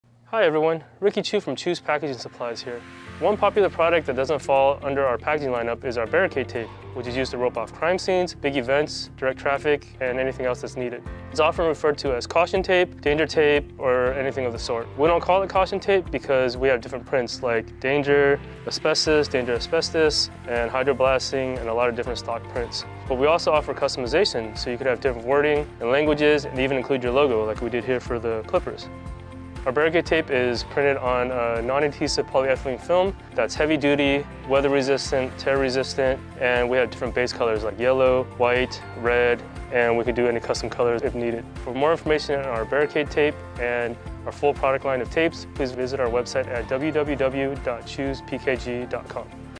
Barricade Tape 101: Everything You Need to Know
In this video, Ricky Chu from Chu's Packaging Supplies shares his expertise on barricade tape, a popular product used for safety and security in various settings. From crime scenes to construction sites, barricade tape is essential in alerting people of potential danger, directing traffic, and creating a secure boundary.

Ricky covers everything you need to know about barricade tape, including the different prints available, the heavy-duty materials used, and the various base colors that can be customized to fit your specific needs. You'll also learn the difference between caution tape and danger tape, and why the non-adhesive polyethylene film used in barricade tape is weather-resistant and tear-resistant.

Whether you're a business owner looking to promote your brand, or an event organizer searching for the best way to keep attendees safe, this video has all the information you need to make an informed decision about using barricade tape.

So if you want to learn more about this versatile and essential product, watch this video now and discover how barricade tape can help keep you and your community safe and secure.